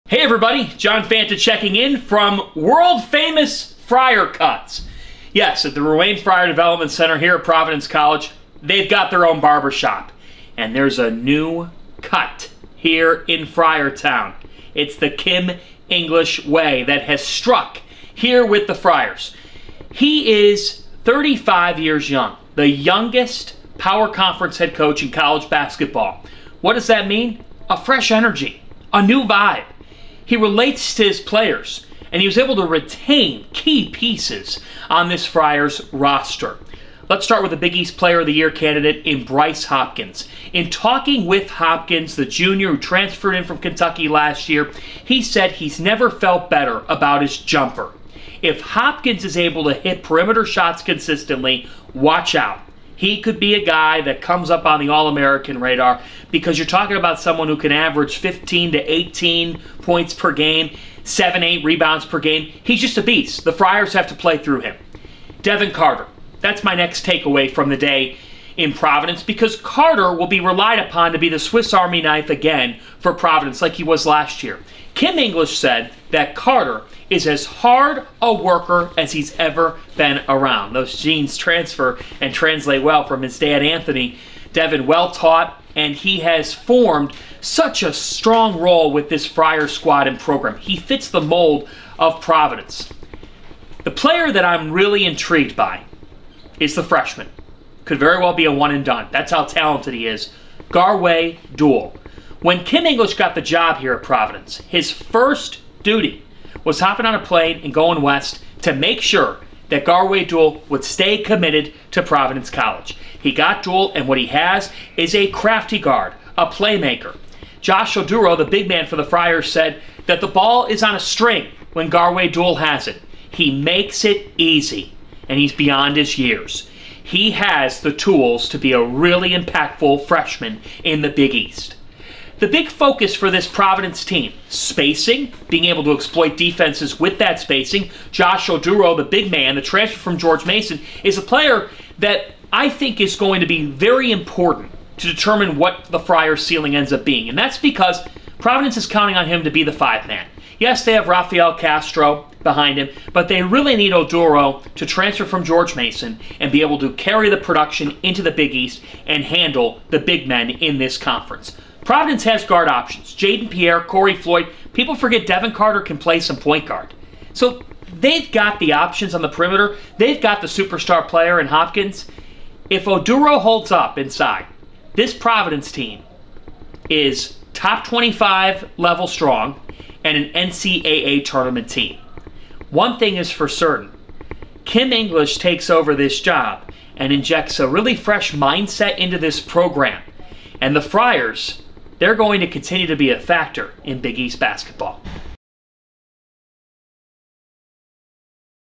Preseason Tour: Providence Friars Basketball
📍 Providence, RI

Our Preseason Tour kicked off today in Friartown! John Fanta tells you all you need to know about the upcoming season for Providence.